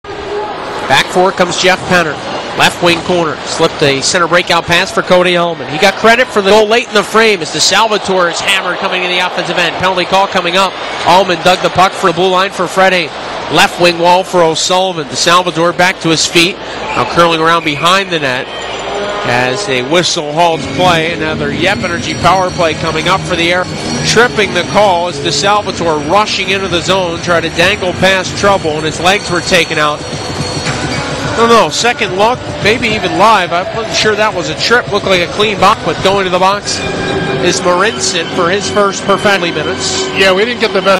Martin Marincin first pro (AHL) two penalty minutes, tripping
From AHL game (Oklahoma City Barons (Marincin) @ Houston Aeros/ Sunday, April 10, 2011